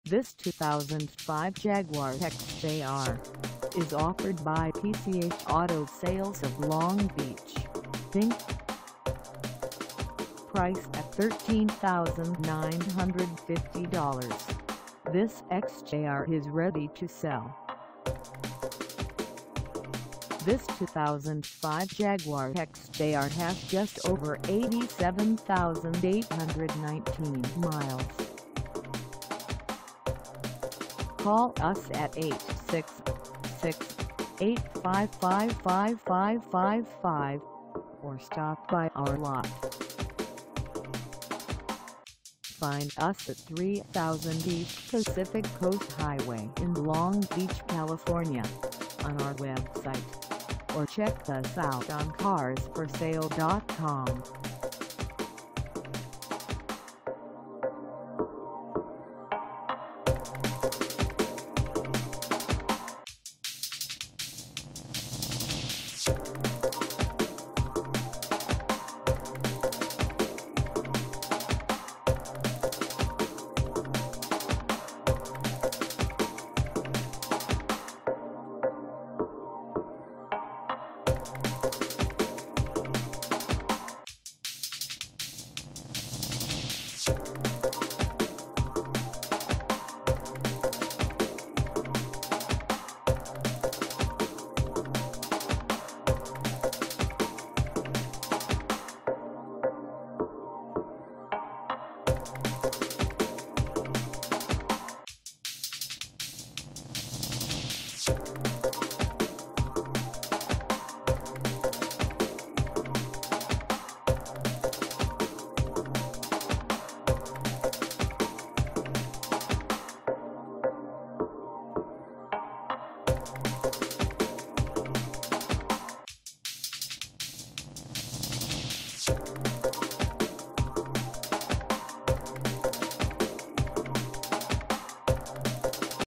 2005 Jaguar XJR for sale in Long Beach, CA 90804 at the PCH
This 2005 Jaguar XJR is for sale in Long Beach, CA 90804 at PCH Auto Sales of Long Beach, Inc.
Engine: V8 4.2L V8
Transmission: Automatic 6-Speed
Color: WHITE
Mileage: 87819
Contact PCH Auto Sales of Long Beach, Inc 3000 East Pacific Coast Highway Long Beach, CA 90804 to test drive this 2005 Jaguar XJR  today.